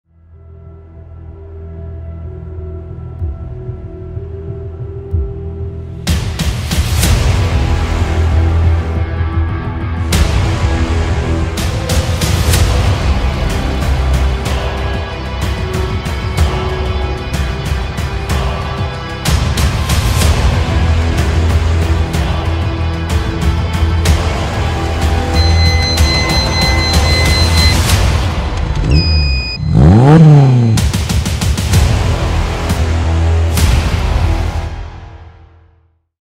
The Absolute Sleeper - TRAILER
A new monster to the channel ! A Honda Civic F20B with a lot horsepower under the hood . Soon the full video.
🔻Special thanks 🔻
Car owner: